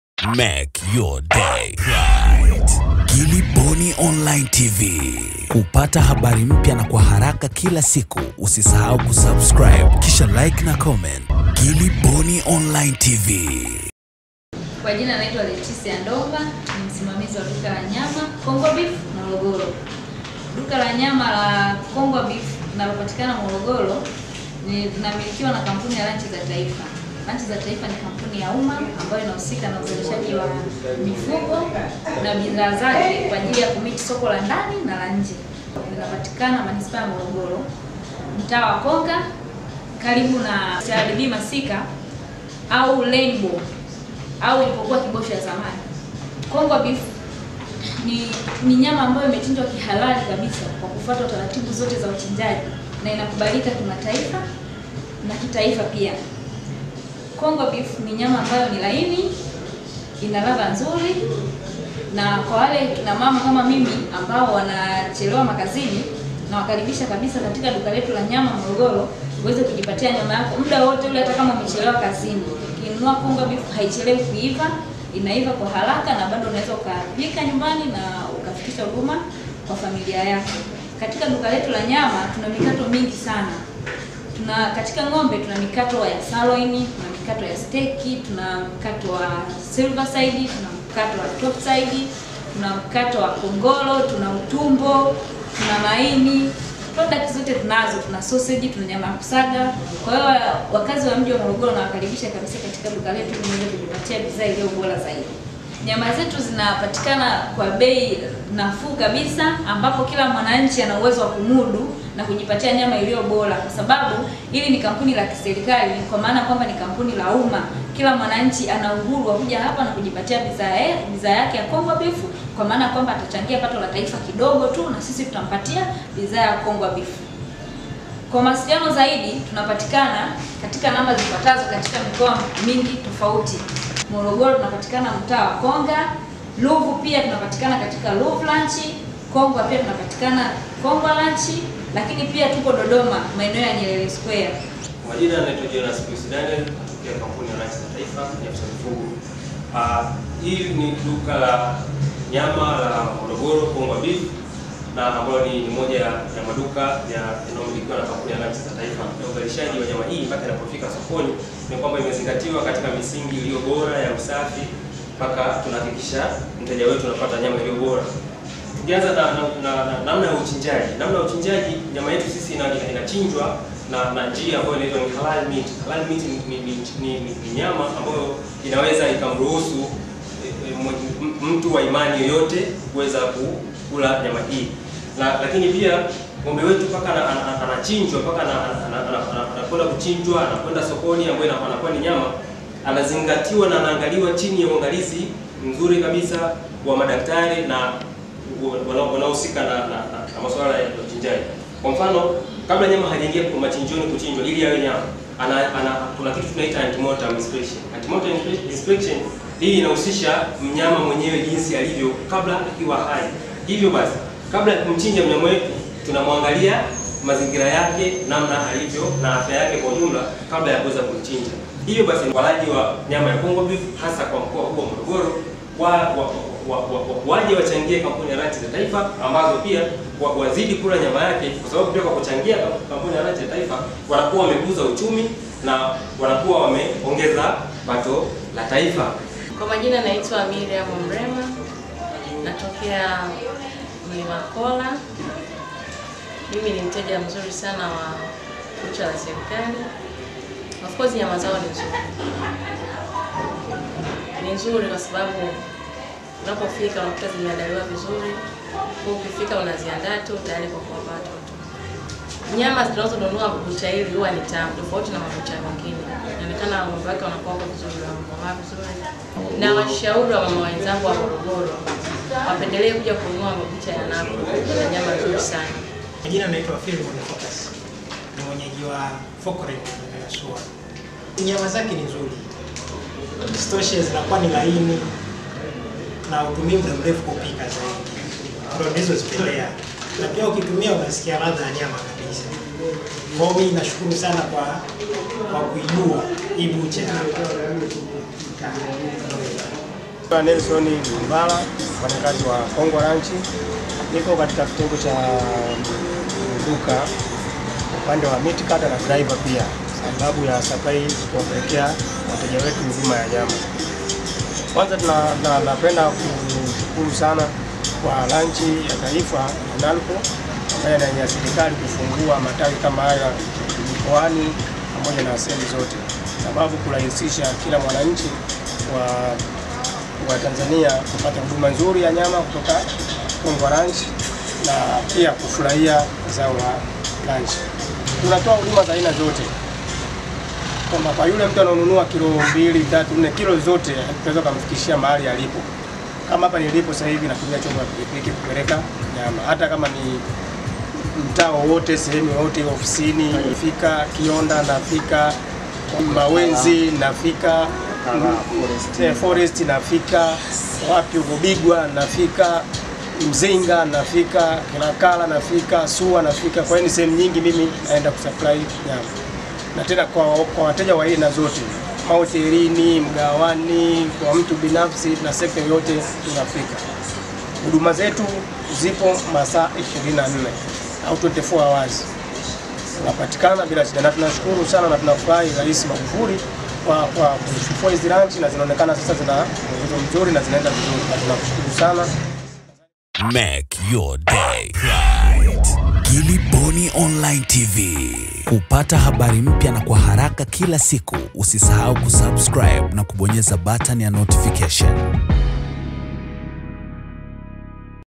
Serikali yaanza kusambaza nyama Morogoro/ NARCO kuwafikishia huduma saa 24 wananchi
SERIKALI imeanzisha huduma ya kuwasambazia nyama wananchi hadi majumbani kwao baada ya kufungua duka jipya katika Manispaa ya Morogoro linalomilikiwa na Kampuni ya Ranchi za Taifa (NARCO) ili kurahisisha upatikanaji wa huduma hiyo kwa haraka na viwango vya juu vya ubora.